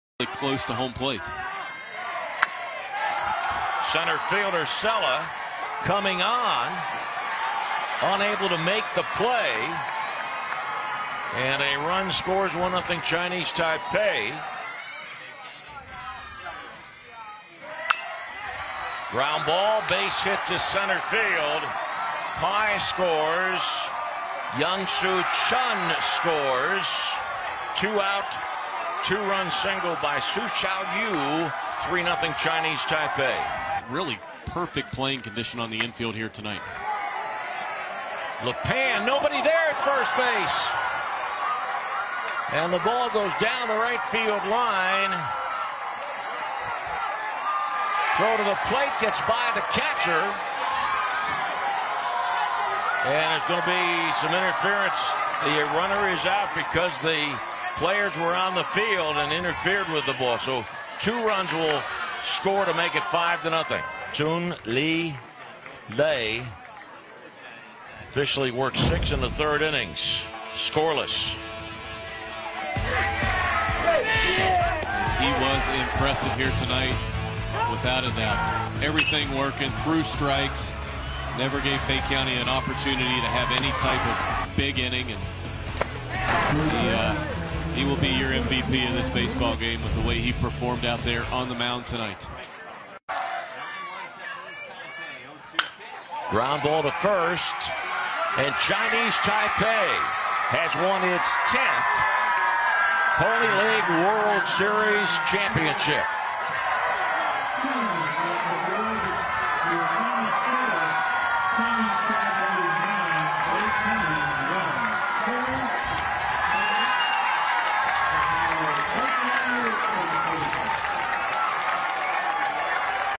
CHAMPIONSHIP GAME: Taipei City, Chinese Taipei- 9 Bay County, MI- 1
2019 DICK’s Sporting Goods PONY League World Series Championship Game
MLB.com